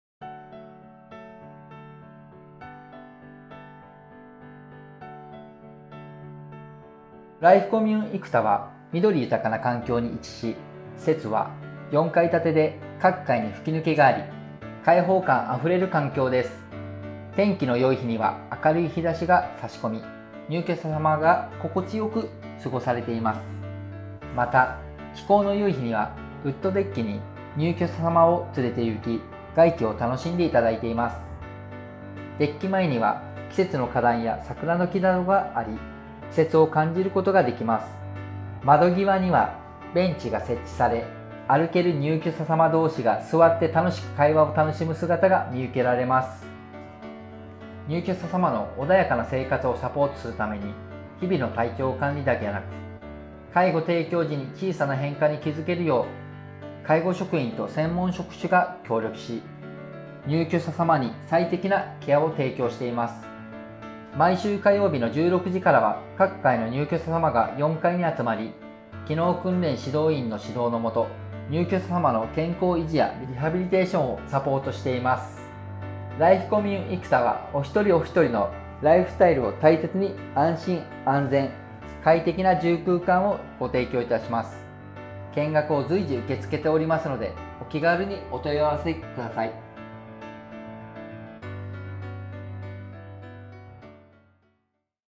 「ライフコミューン生田」の施設案内 - 介護付き有料老人ホーム【いい介護】
「ライフコミューン生田」の周辺には緑豊かな生田緑地があり、四季折々の自然を感じることができます。施設内にも心を癒してくれるような数多くの空間が。さらに、介護設備や介護サービスも充実。安心して、穏やかなシニアライフを送ることのできる介護施設です。

■入居時費用：0.0万円〜
■月額費用　：16.0万円〜
■入居条件　：自立・要支援・要介護・認知症可・認知症重度可
■施設住所　：神奈川県川崎市多摩区三田4-5555-1
■アクセス　：小田急小田原線「生田」駅南口より徒歩12分（約960m）
■運営会社　：株式会社木下の介護

チャプターリスト
0:00 イントロダクション
0:07 施設周辺と住環境
0:25 ウッドデッキ・ベンチ
0:47 人員体制
1:04 機能訓練指導員によるリハビリテーション
1:25 見学のご案内

今日明日の見学予約も受付中！　土日祝日も見学できます！